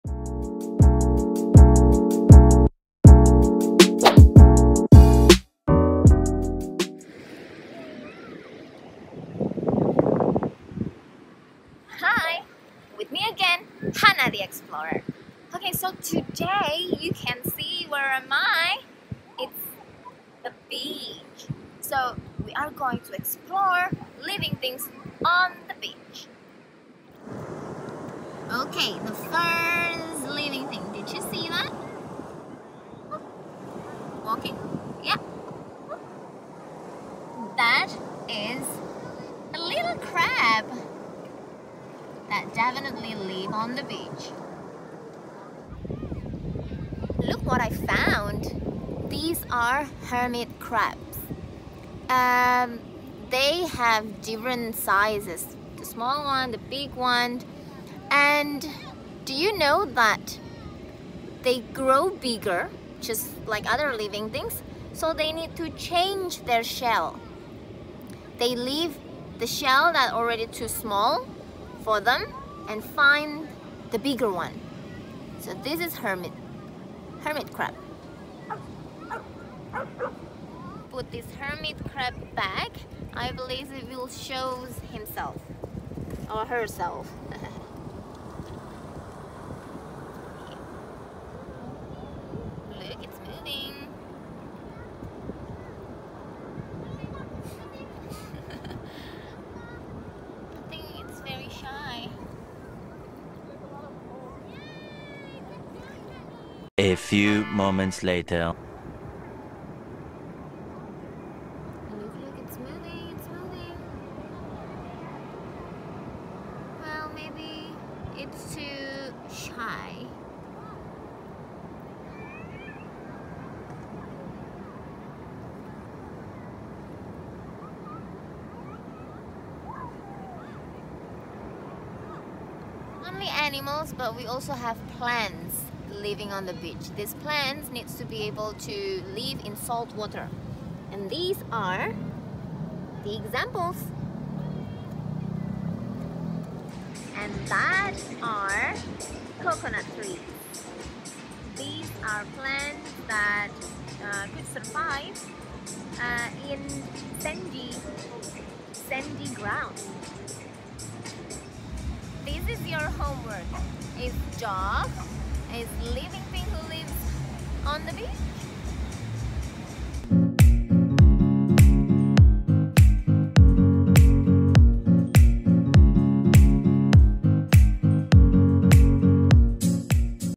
Living Things and Their Environment - Beach
What can we find on a beach?

Hana the explorer will show us what are living things that we can find on the beach. There are different types of animals and also plants.

Let's go!